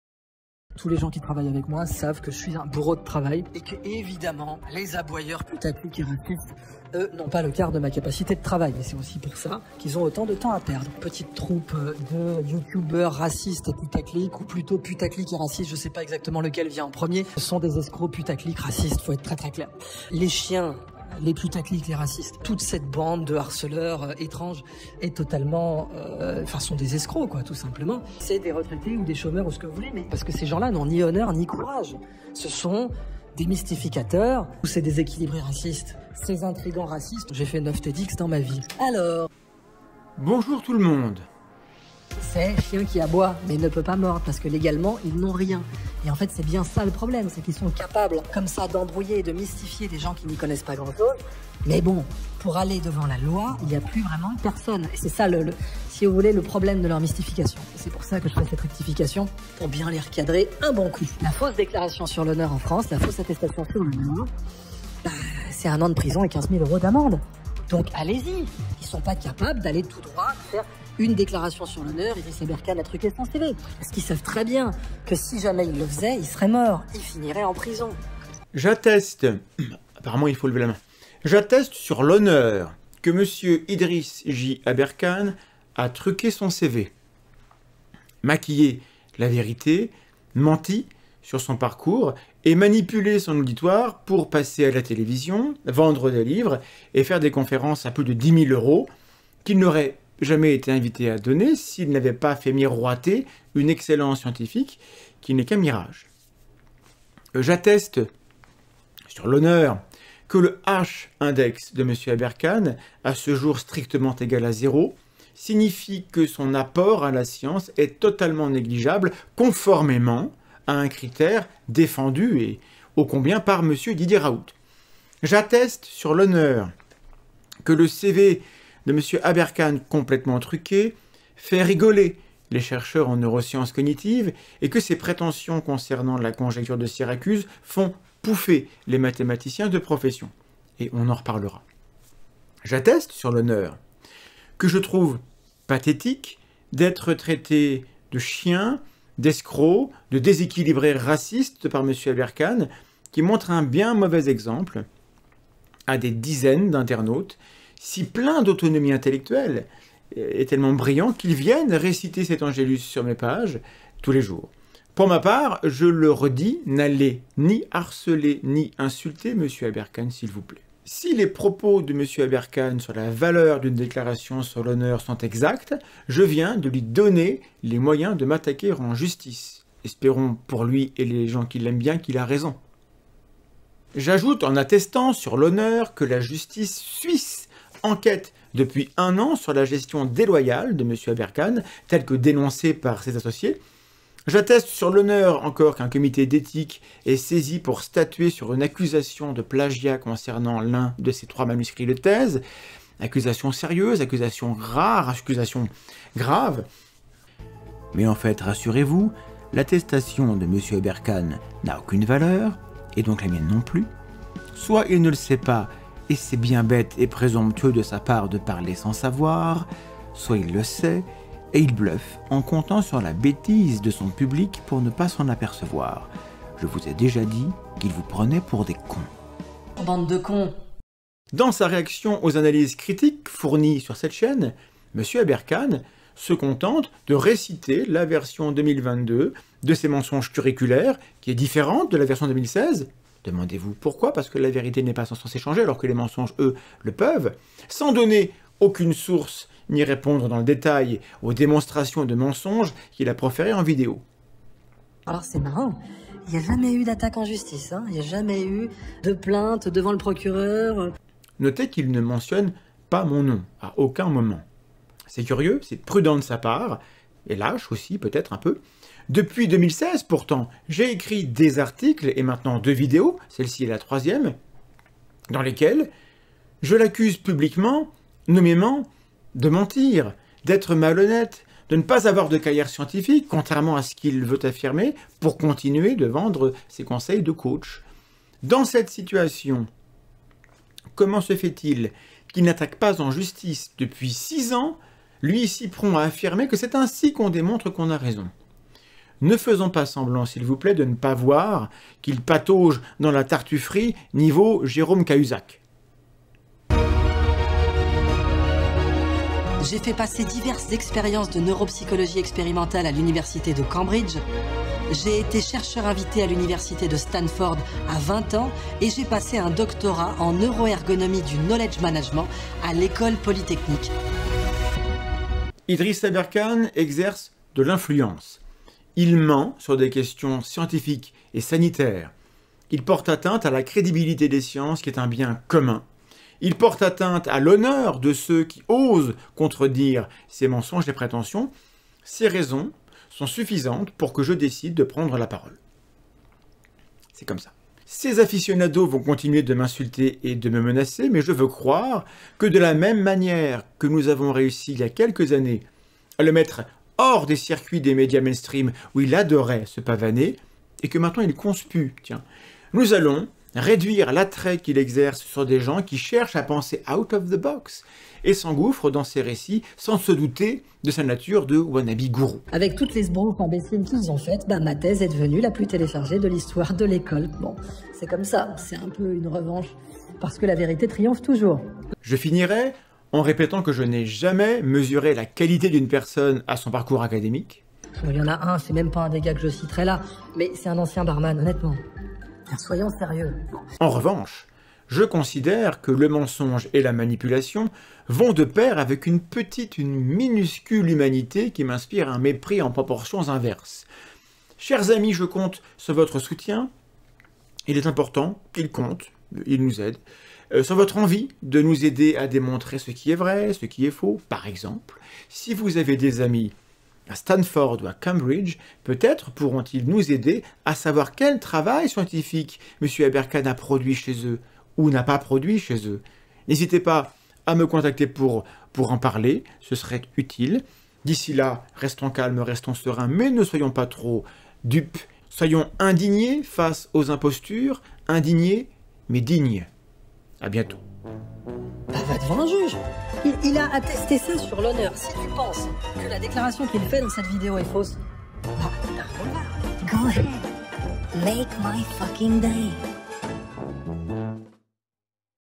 Le bluff et le déshonneur d'Idriss Aberkane
En réponse à nos deux vidéos démontant sa rhétorique manipulatoire et le trucage de son CV, Idriss Aberkane a répondu, sans me nommer dans « Je BALAYE quelques youtubeurs racistes et putaclic » par plus de mensonges (sur la nature et la valeur de ses publications et de ses stages) par l'esquive (sur la composition de ses jurys, sur la non reconnaissance d'un de ses doctorats, sur son statut "d'officier de marine") et par l'injure : nous sommes des chiens,  des escrocs, des déséquilibrés racistes, des intrigants racistes.

Sa nouvelle invention : une attestation sur l'honneur en vertu de laquelle il est  désormais considéré comme ayant toujours dit la vérité jusqu'à ce que quelqu'un porte plainte contre lui. Il s'agit d'un bluff, et cette vidéo vous le prouve.

Merci beaucoup à ceux qui soutiennent déjà le travail de la Tronche en Biais via les plateformes de financement :
▲ Et puis, pensez à visiter donc la boutique de la TeB